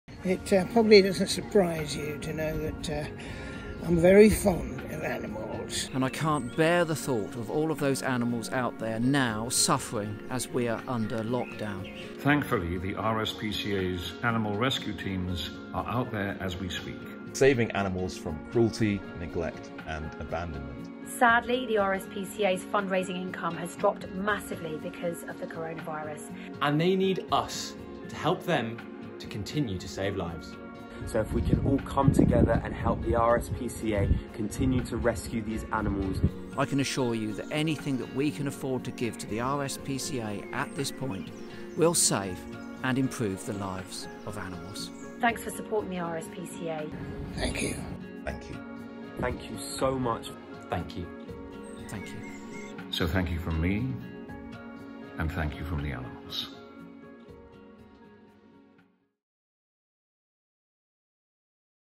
RSPCA Celebrity supporter appeal
Some of the fantastic supporters of the RSPCA have given up their time to come together to ask for your help in supporting the RSPCA during this difficult time.

Thank you to Bill Oddie, Chris Packham, Peter Egan, Patrick Aryee, Kate Lawler, Rory Cowlam and Eyal Booker for helping us with this video!